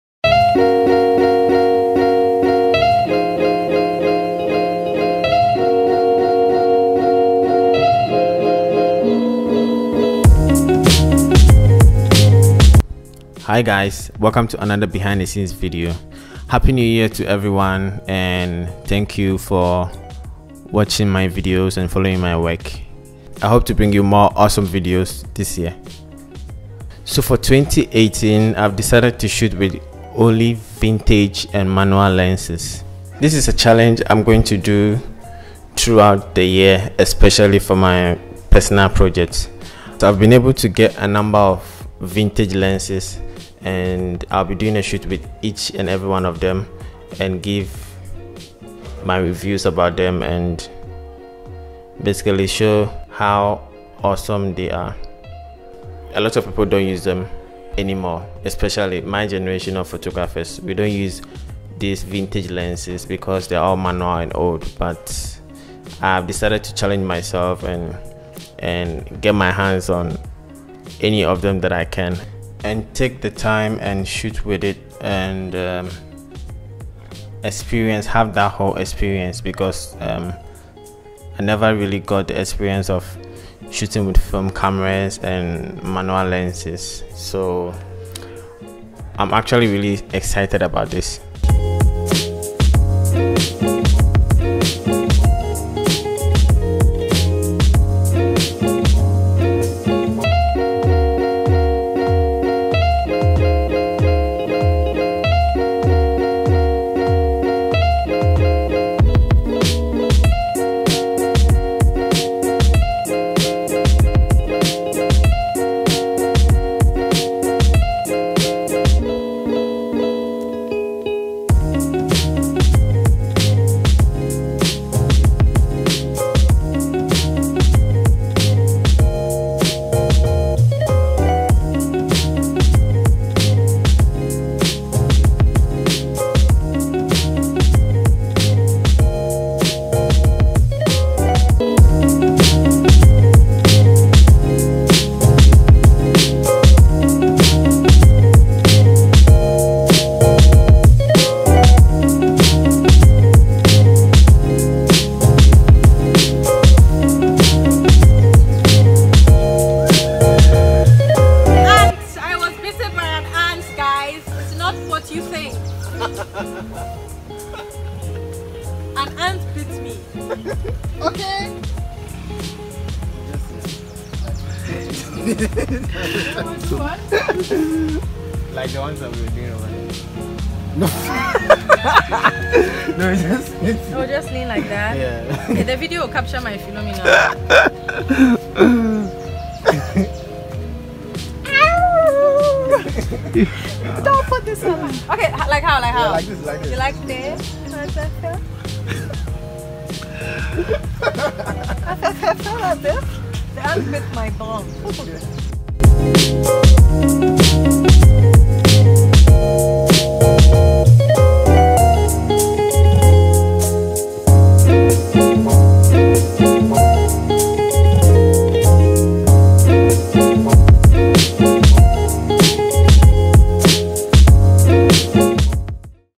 CHALLENGE: Vintage and Manual Lenses ONLY!!! (Fujifilm X-T2 and Helios 44-2 58mm f2)
Happy New Year to all of you guys. Thanks for your views.
2018 is going to be an awesome year!!!
I got inspiration from a fellow photographer to start shooting with vintage and manual lenses. I've managed to get my hands on a couple of vintage lenses and i'll be shooting with them through the year. There are a whole lot of great old lenses that young photographers are not using or exploring.
Initially manual lenses were such a turn-off for me...but using it with my Fujifilm X-T2 makes it much easier for me to use with the help of focus peaking.
Looking forward to do more shoots!!!

GEAR USED:
Fujifilm X-T2
Helios 44-2 58mm f2
Godox AD360
Selens 120cm deep octabox